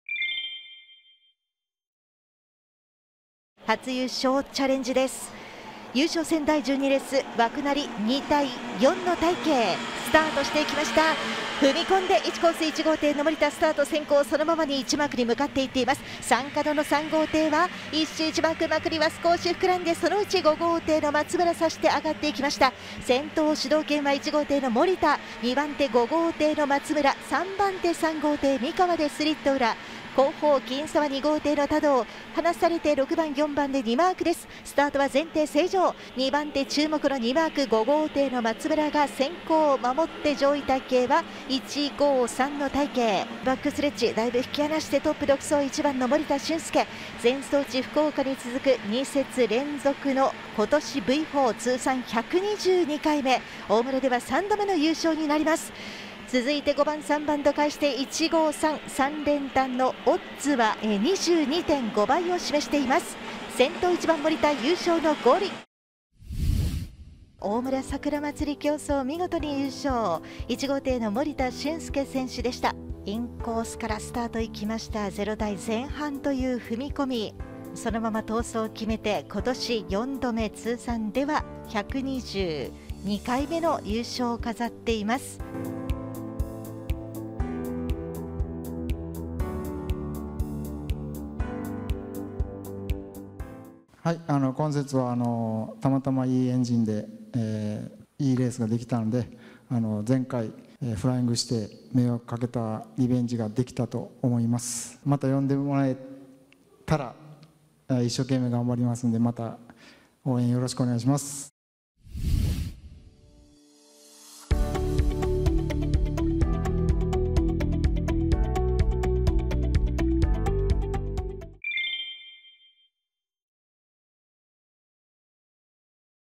守田俊介 通算122回目の優勝！ 12戦11勝 準パーフェクトV達成！│BOATCAST NEWS 2024年4月1日│
守田俊介選手、通算122回目の優勝！ 12戦11勝 準パーフェクトV達成！
大村一般 最終日 12Rをご覧ください！